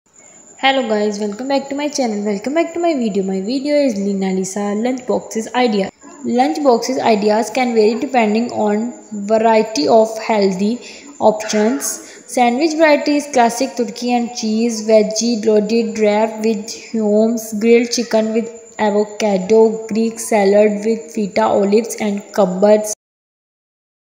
lisa lena#lunch box ideas#@lenalisacollection4818
Hy frnds!

Pasta Salad: Prepare a pasta salad with whole-grain pasta, cherry tomatoes, cucumbers, and Italian dressing
Pack carrot, cucumber, and celery sticks with a side of hummus for dipping.

Leftovers: Utilize dinner leftovers like grilled chicken, roasted vegetables, or rice to create a tasty lunch.